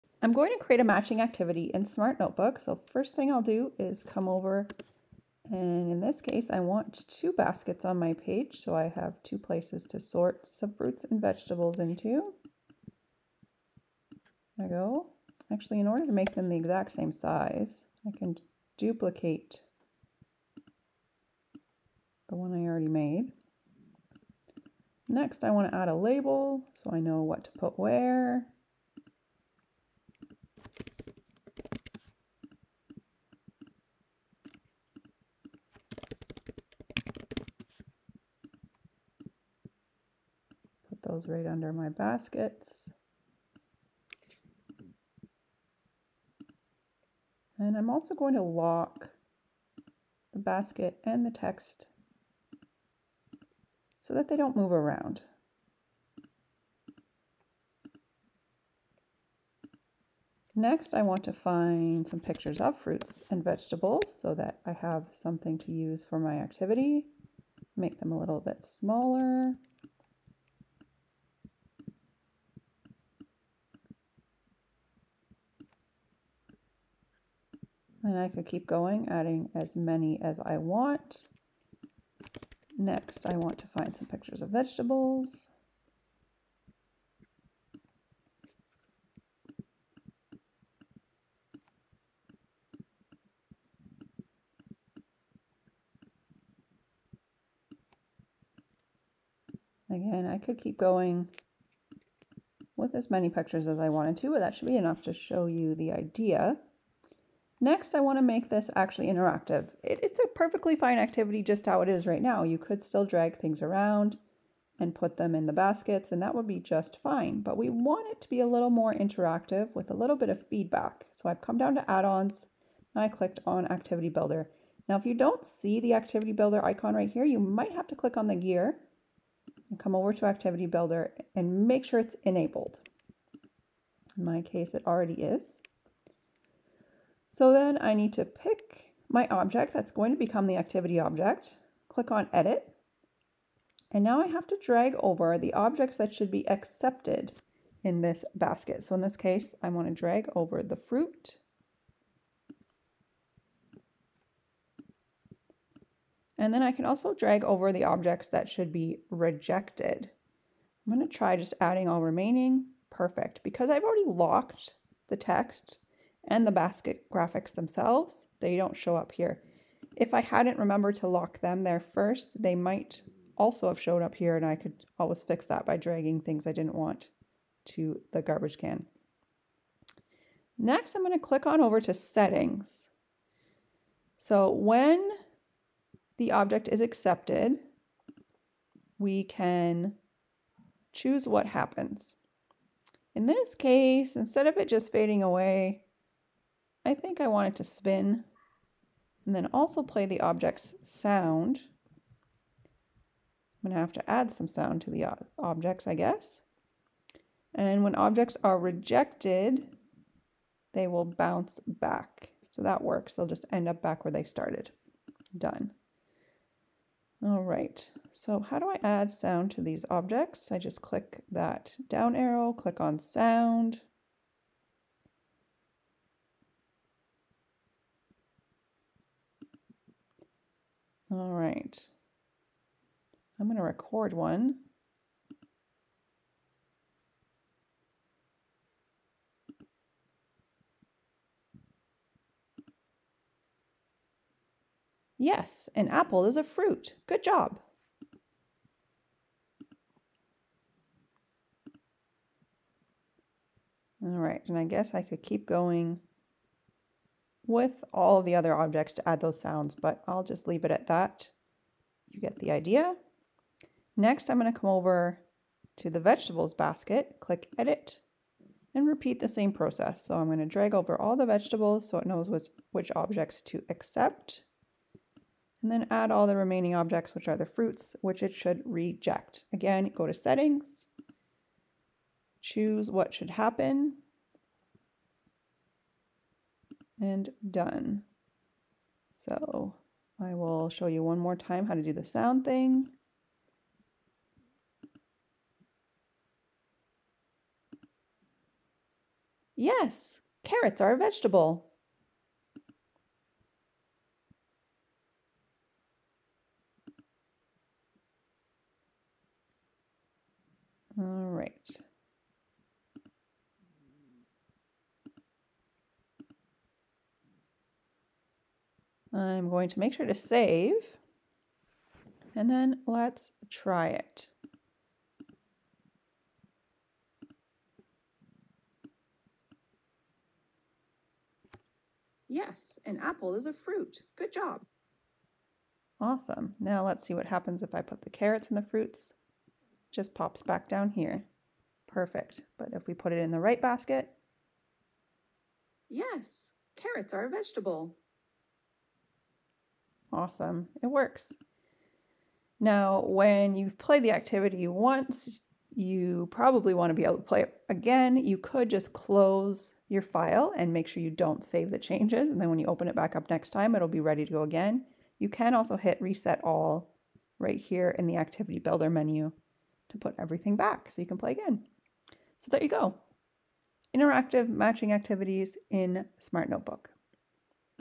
Creating Custom Interactive Matching Activities in SMART Notebook
You can find all kinds of wonderful interactive activities for SMART Notebook, but sometimes you just need to create your own! In this quick video, I show you how to add graphics from the gallery (you could also import your own from anywhere), then use the activity builder add-on to designate interactive objects and set up a custom matching activity. In this case, I demonstrated sorting into two categories - but your activity could have 1, 2, 3 or however many objects you liked - it's completely up to you! I also show you how to add custom sound, which lets you build in feedback for your students.

Hope this helps! If there are other things in SMART Notebook you would love to know more about, let me know!